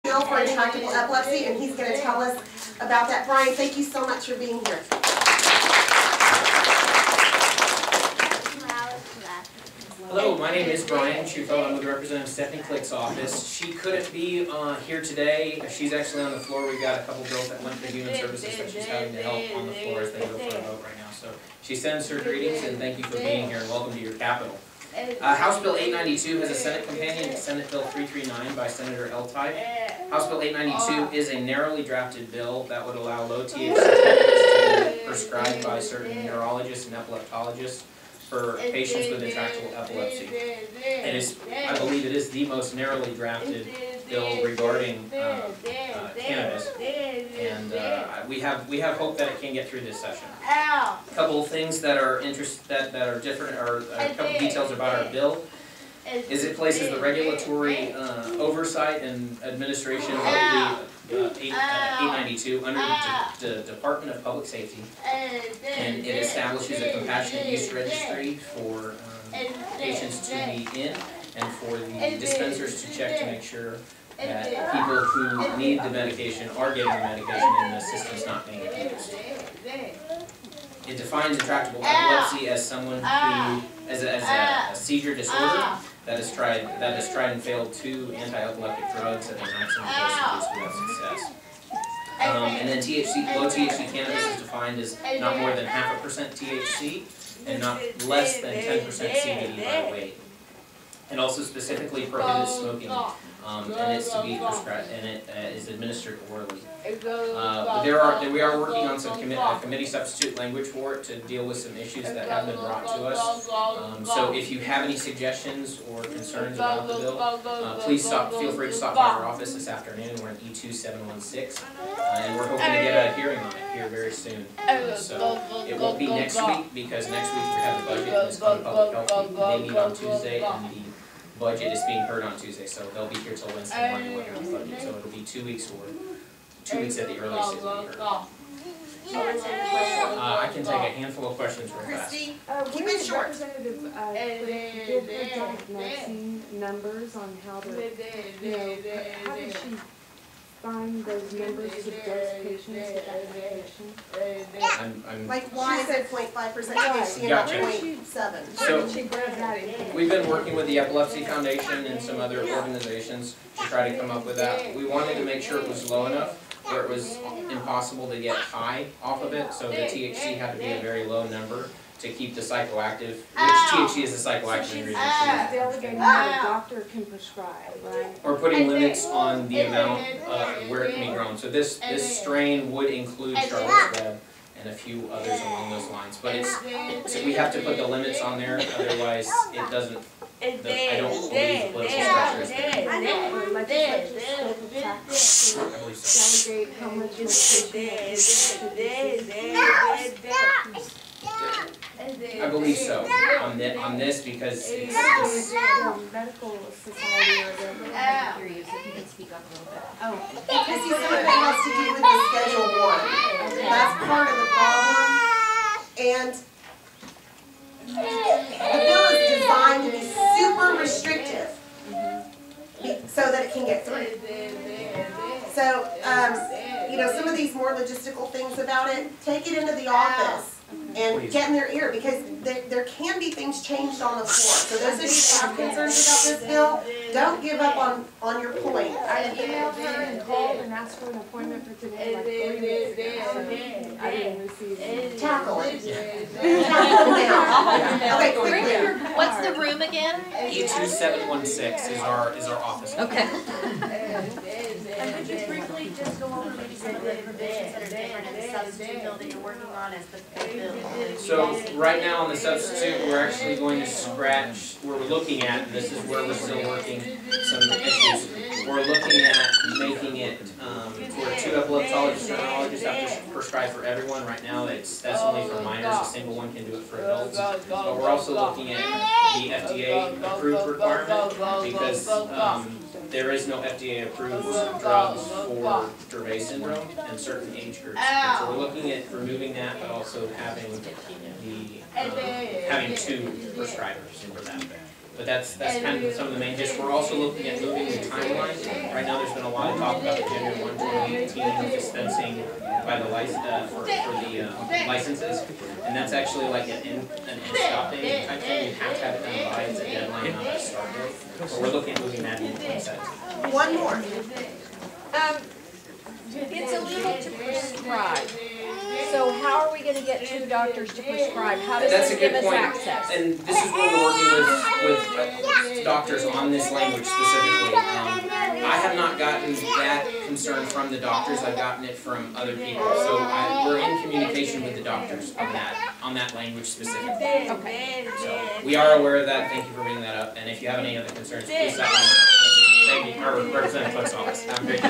Bryan Policy Analyst from Stephanie Klick Office on HB 339
Bryan speaking for Stephanie Klick Office on Texas HB 339 at MOMS LOBBY DAY on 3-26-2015.